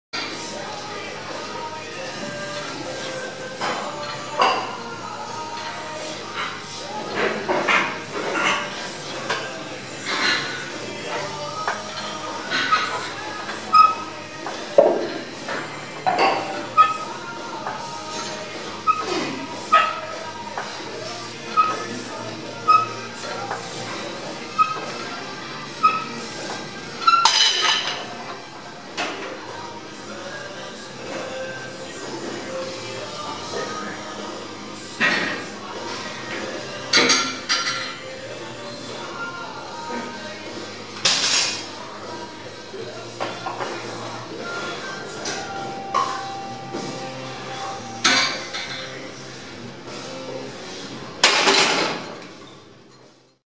Kevennetyt leuat + Meadows-vinopenkki | 7.11.2015
Selkää ja rintaa supersarjoina, 12-15 toiston settejä.